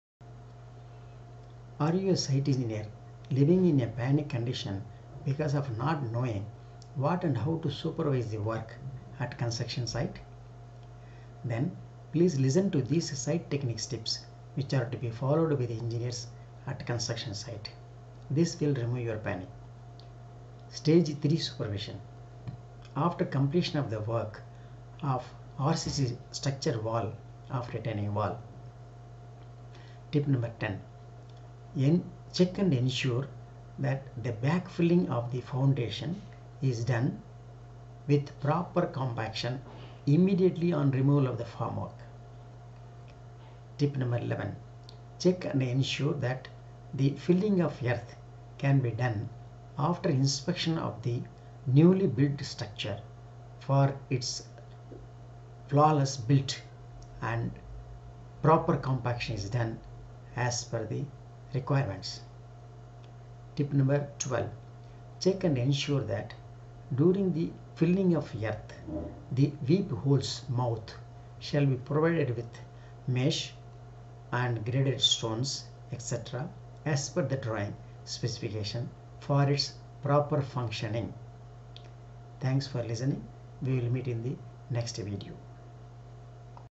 SET-73-Site Techniques TIPS for Engineers- Retaining Wall (RCC Structure wall – toe/heel/stem)

Are you living in panic at Construction site, because of not knowing what and how to supervise the works at site?

Then, please listen to these TIPS of the Site techniques which will remove your panic.

Your feedback on this is highly solicited for further TIPS and clarifications.